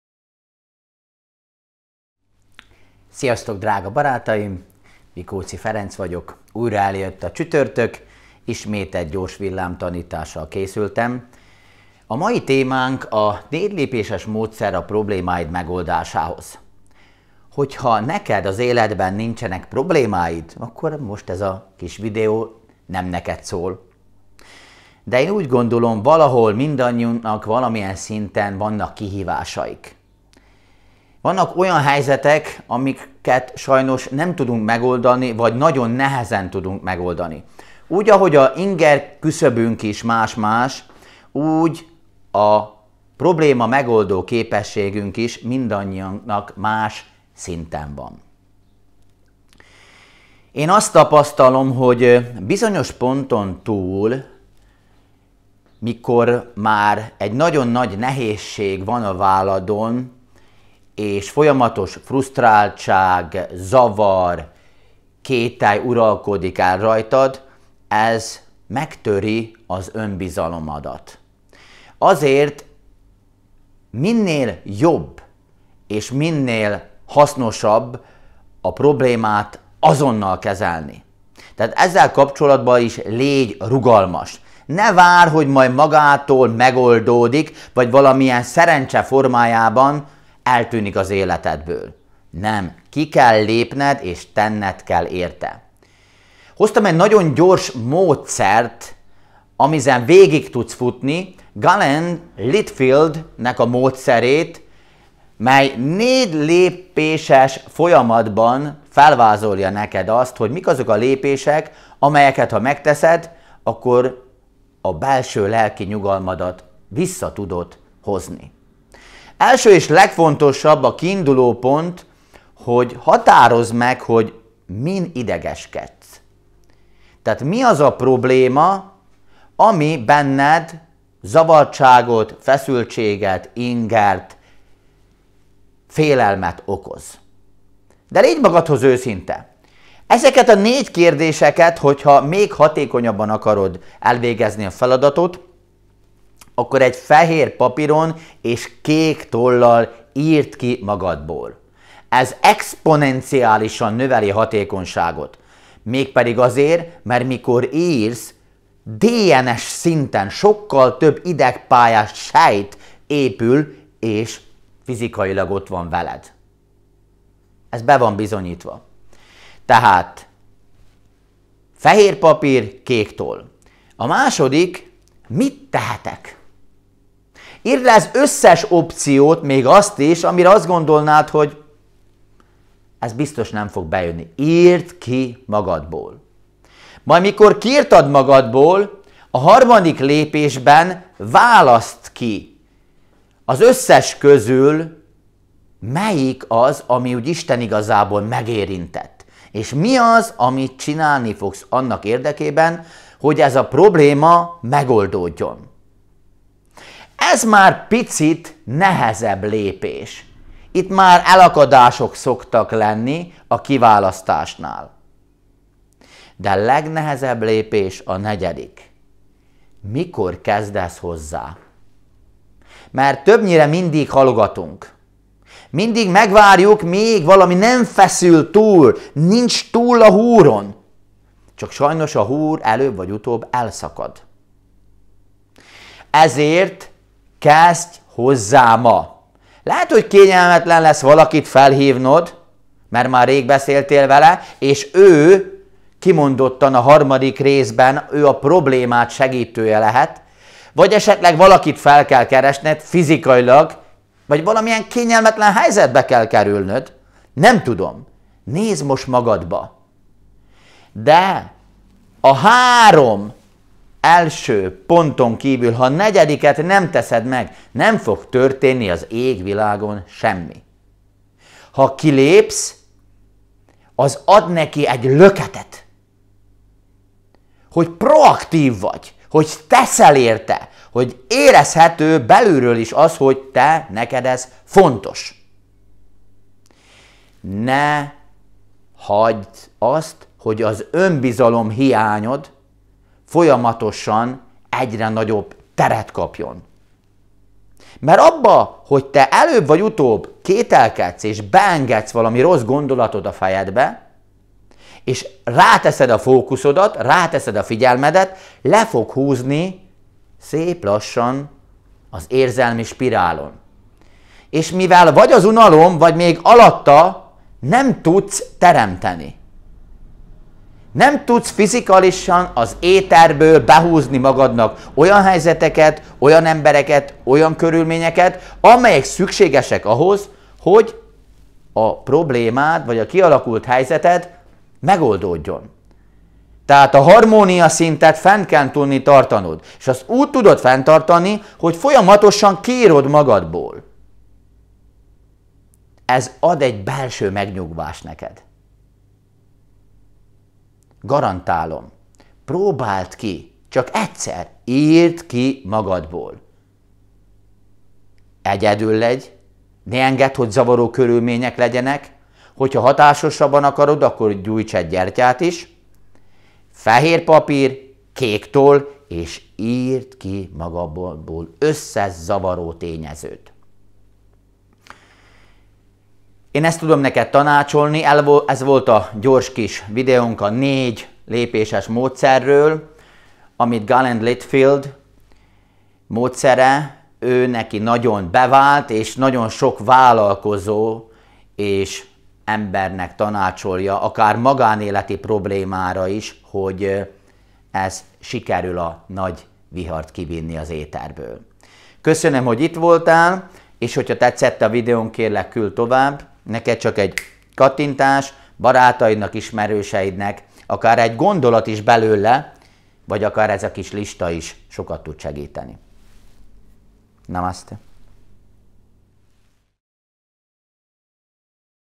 Mikóczi Ferenc: 4 lépéses módszer a problémáid megoldásához
A régi szokásunkat folytatva, a hét negyedik napján ismét elhoztam neked a legújabb villámtanításomat. Ami, mondjuk úgy, egy kis útmutató, amelyben Galen Litchfield problémamegoldásra kifejlesztett 4 lépéses módszerét mutatom meg neked.
Namaste!